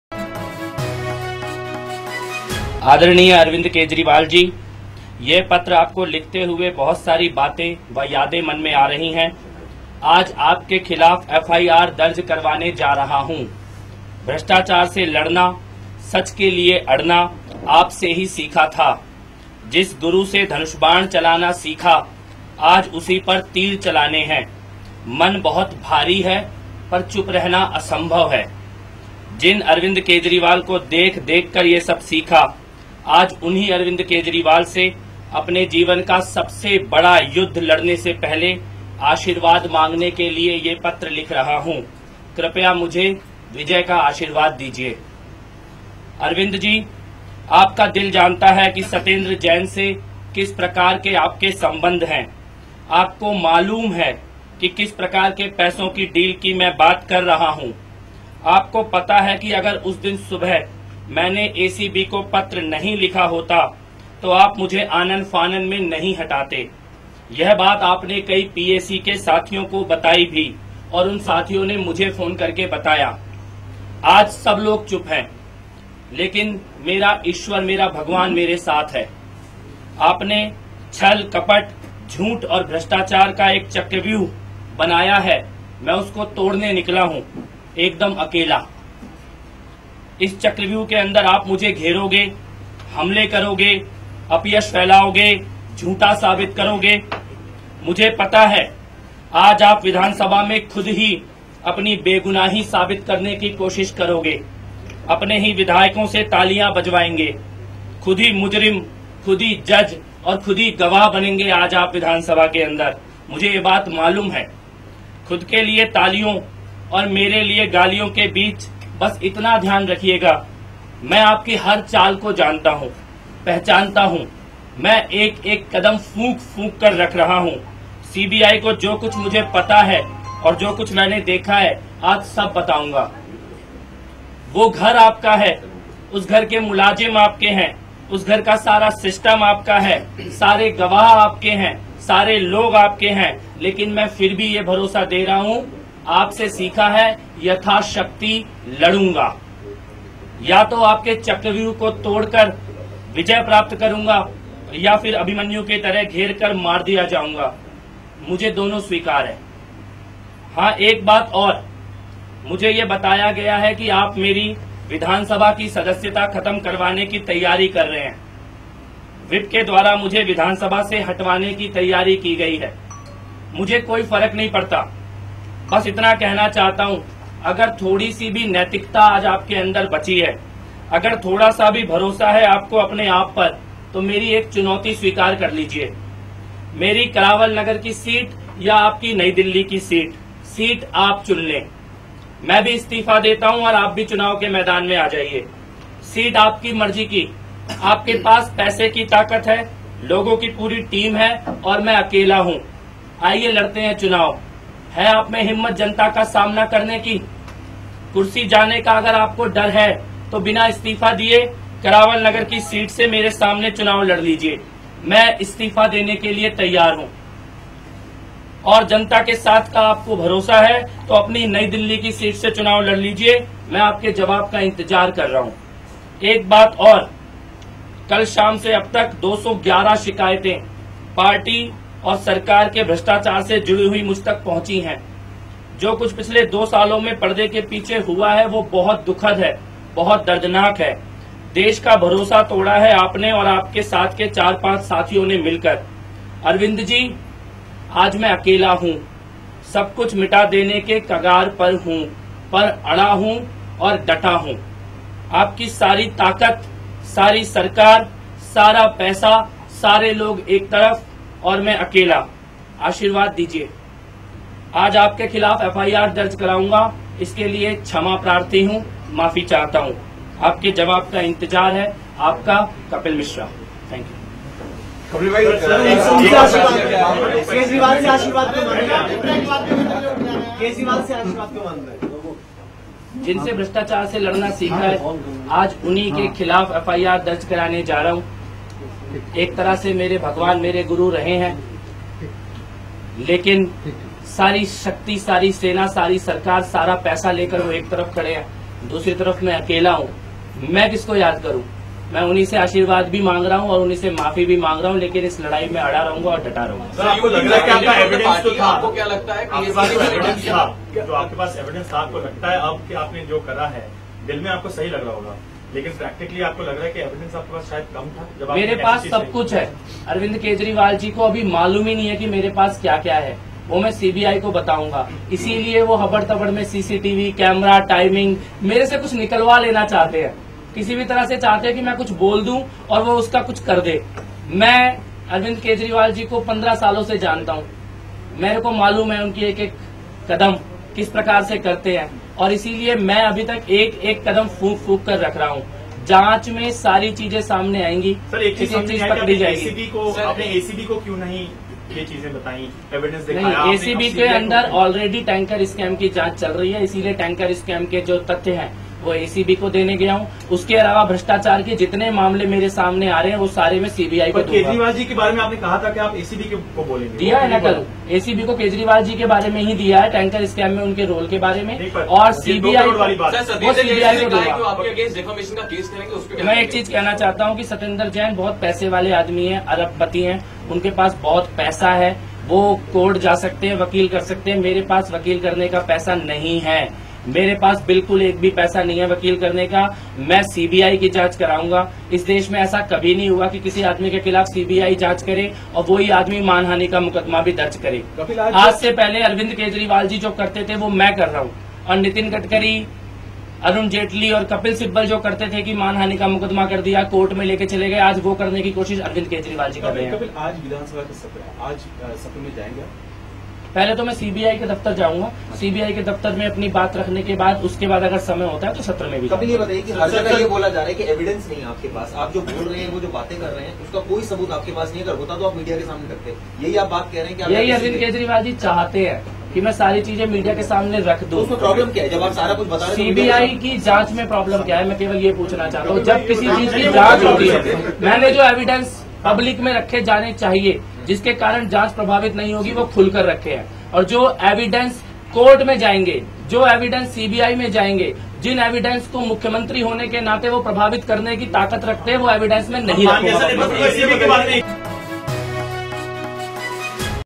Will file FIR against Arvind Kejriwal: Kapil Mishra
Sacked Aam Aadmi Party (AAP) minister Kapil Mishra has announced on Tuesday morning that he will file FIR against Delhi Chief Minister Arvind Kejriwal.

Mishra, who accused Kejriwal of bribery, read out an open letter to Delhi CM in front of the media on Tuesday.

"You know the truth. I know you will attack me, frame me and try to trap me in a chakravyuh of lies and deceit"," he read.

Mishra also told reporters that he will reach the CBI office at 11:30 am where he will make a formal complaint against Mr Kejriwal, who he alleged took Rs. 2 crore from another minister, Satyendar Jain, at his Delhi home last Friday.